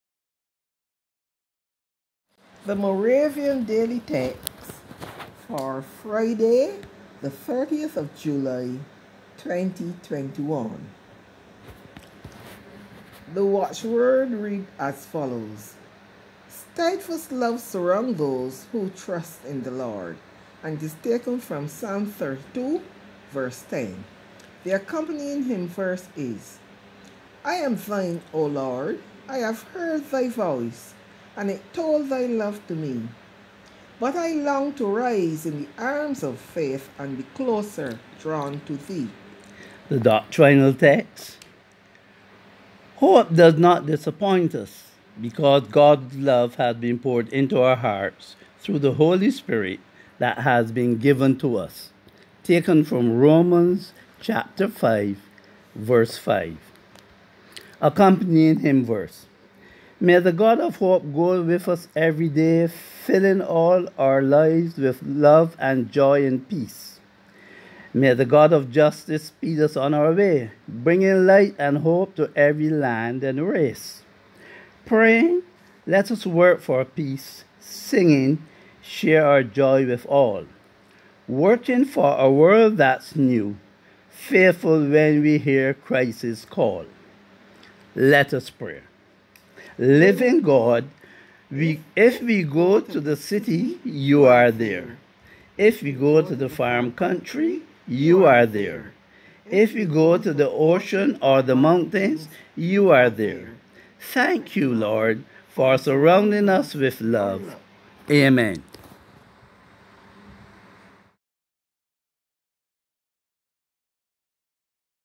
Moravian Daily Texts 2021: Friday July 30, 2021
Today's reading is done by Bro. Maurice & Sis. Valrie Quintyne.

Celebrating another year of togetherness. Happy Anniversary!